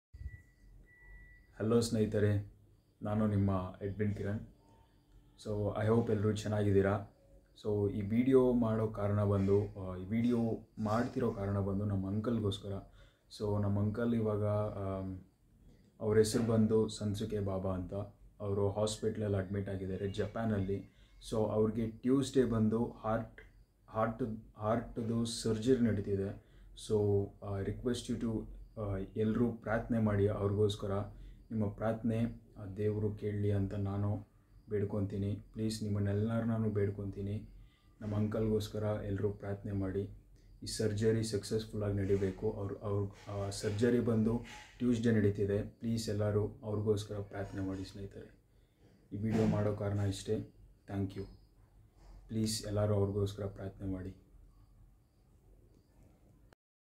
Pray for my uncle Shunsuke Baba
He's been admitted in an hospital for a heart surgery at Japan, I request everyone to pray for home and he's very closest person to me since childhood, Requesting everyone to pray for him.